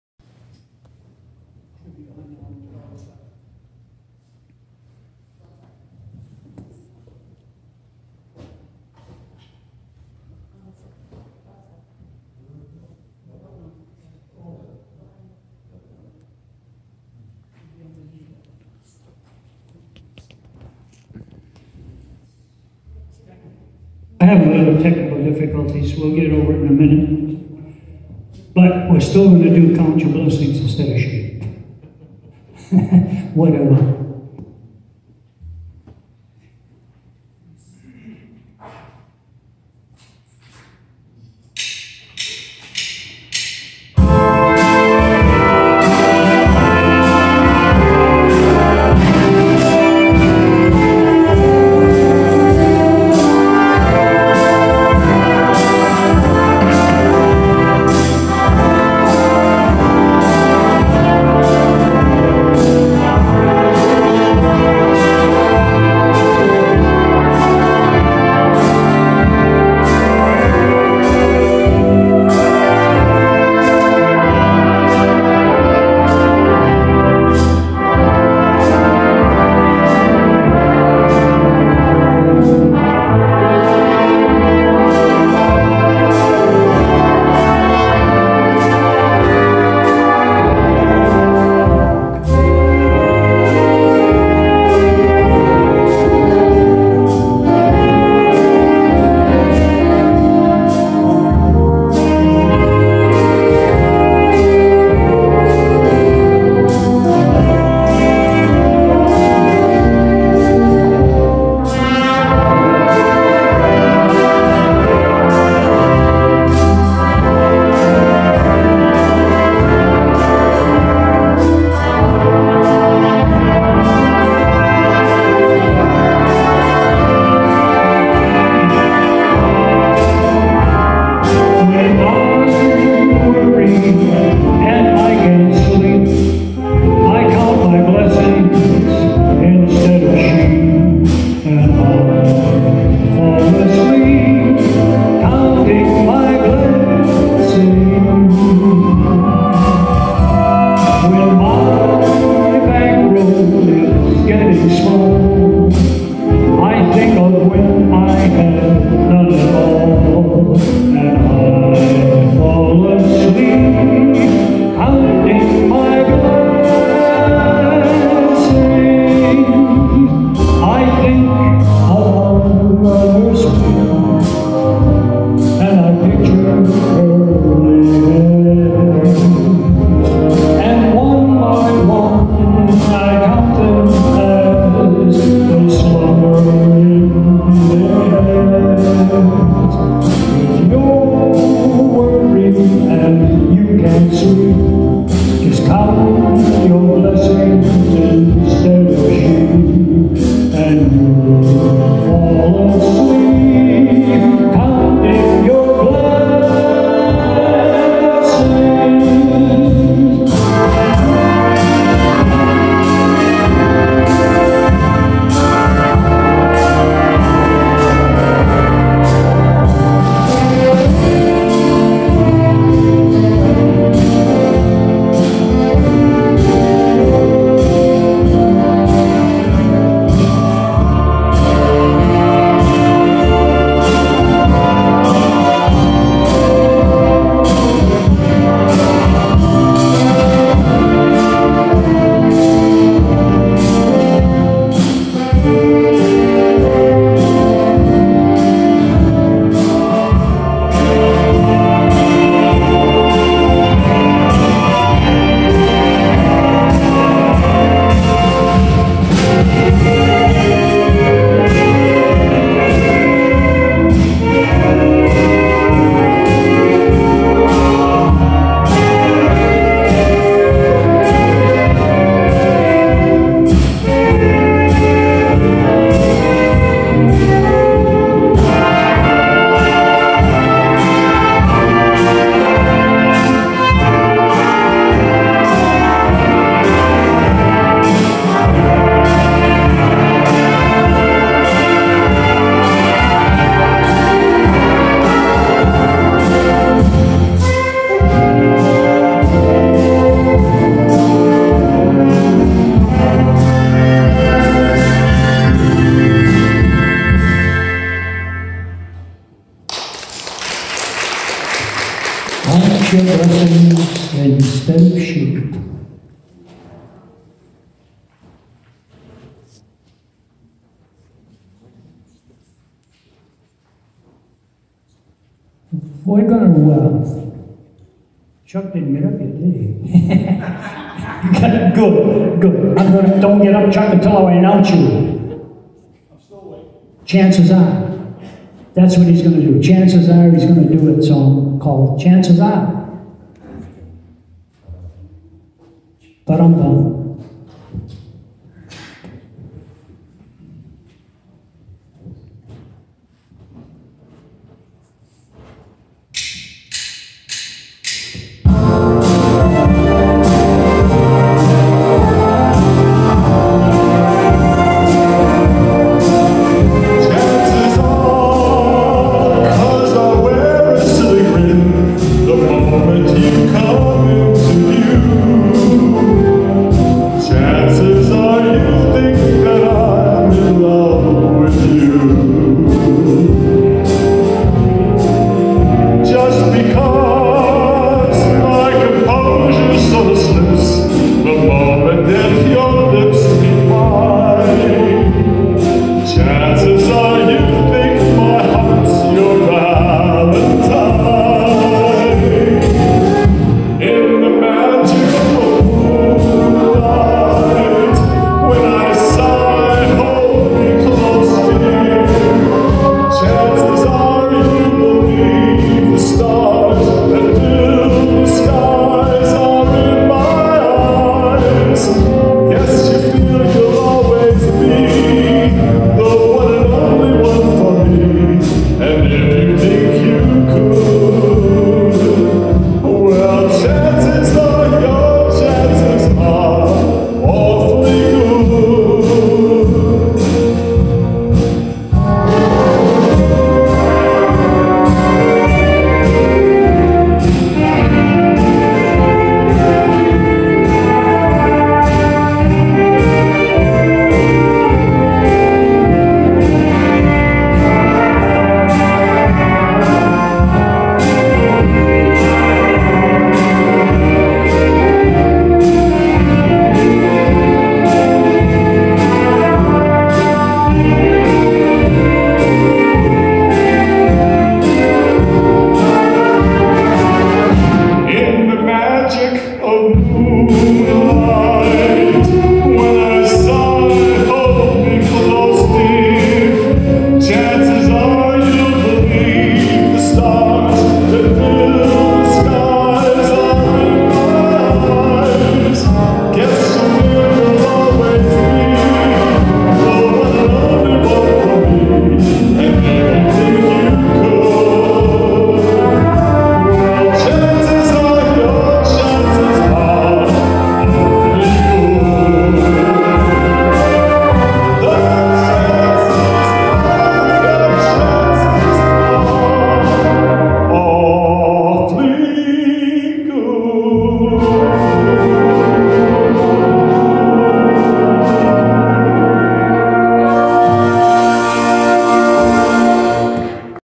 Marty's Reminisce orchestra dancing and desserts 2018 (Orange MA) part 4.
Marty's Reminisce orchestra.
Dancing and desserts
Saturday March 3 2018
Central congregational church.Orange MA.